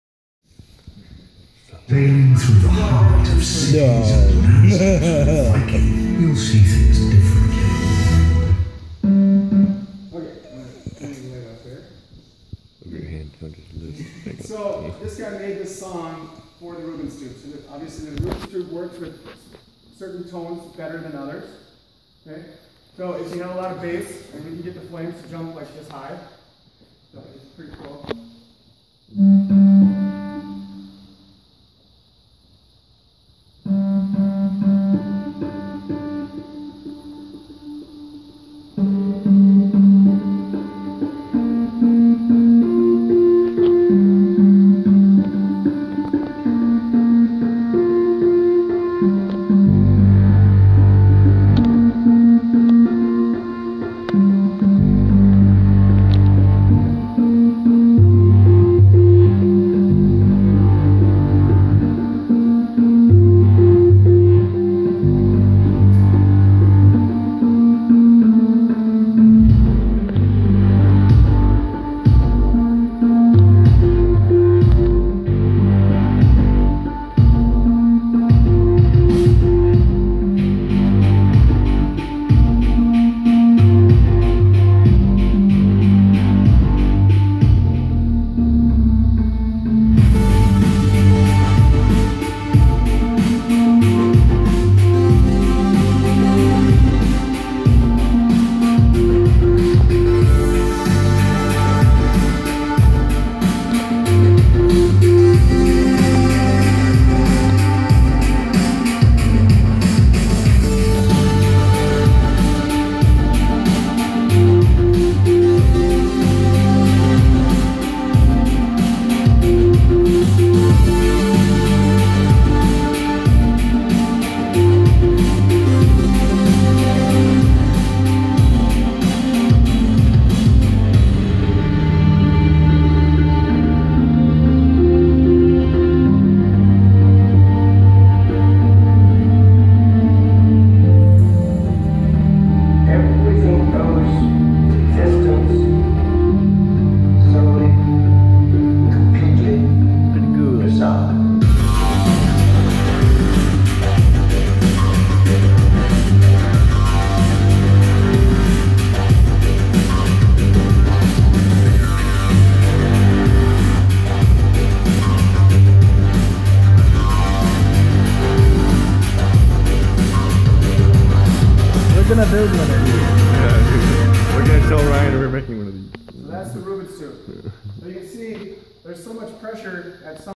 Rubens Tube | NDSCS - Wahpeton, ND(US)
The North Dakota State College of Science got together and made a Rubens Tube: instead of frequency bars that you see on some stereos, this has flame frequency bars. Music plays and the individual flames go up and down to the frequency of the music. Works for any song. I aplogize in advance for not being able to hear the instructors voice very well at the beginning. Enjoy. Please share!

P.S. I have another video of this with a faster pace electronic song, the flames go higher and faster